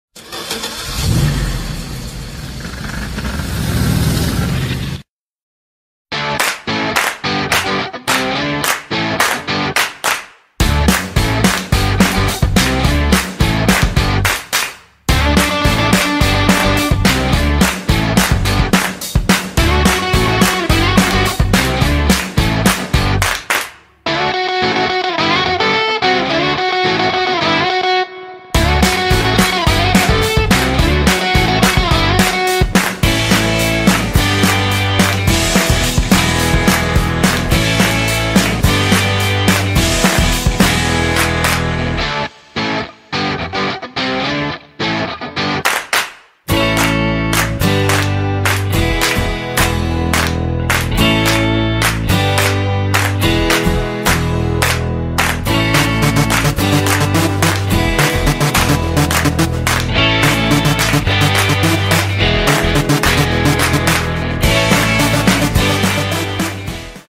He-Man se acerca a los 40 años
En septiembre se celebraron los 37 años del estreno de He-Man y los Amos del Universo, y es por eso que le dedicamos este video.